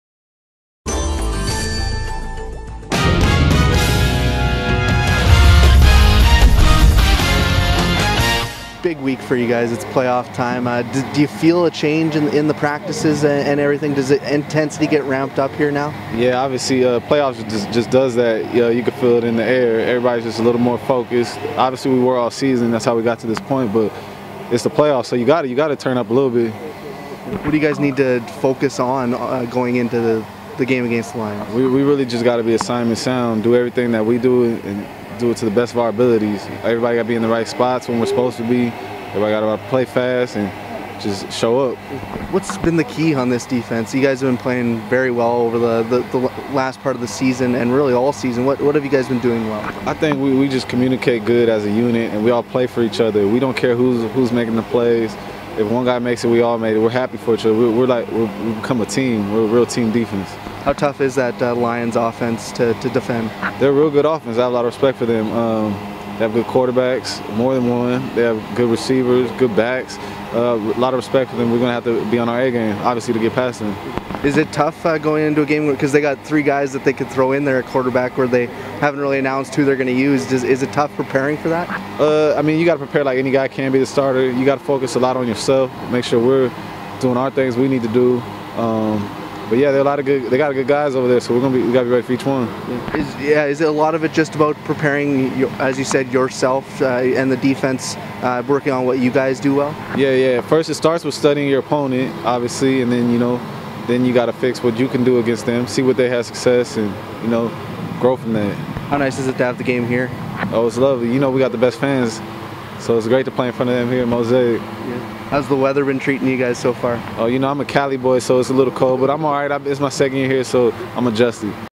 Rider Report - Terrell Maze - Nov 7th
Saskatchewan Roughriders defensive back Terrell Maze talks with DiscoverMooseJaw.com's Marc Smith about the team's preparations for the BC Lions and the West Semifinal.

For more on the Riders, check out the Rider Report on DiscoverMooseJaw.com